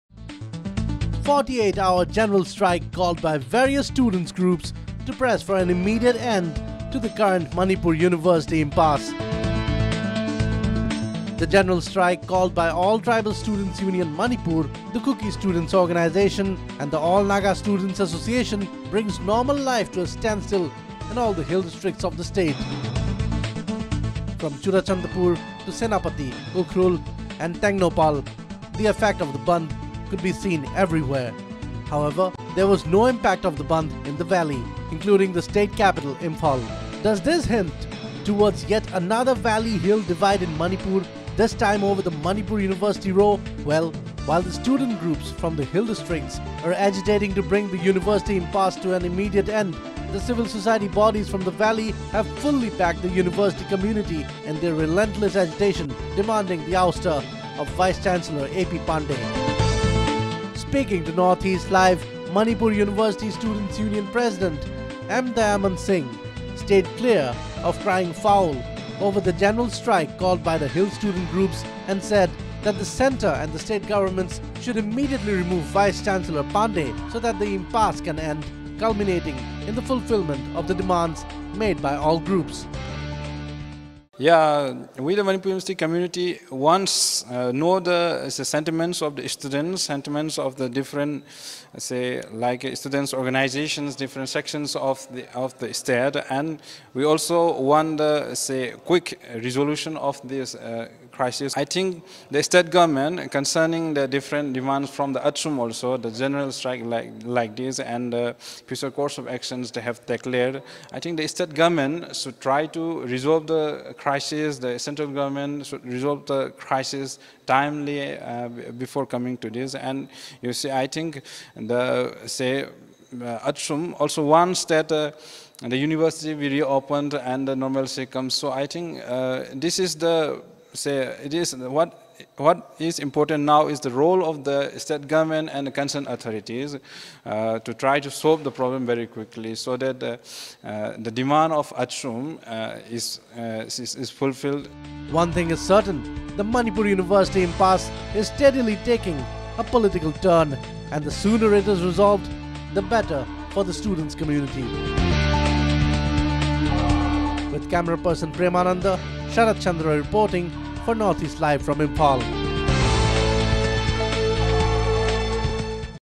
Manipur: 48 hour general strike affects normal life in hill districts
The 48 hour general strike called by the various students groups of the hill districts failed to make any impact in state capital Imphal or in the valley hinting that the Manipur university impasse is yet again dividing the valley and the hills.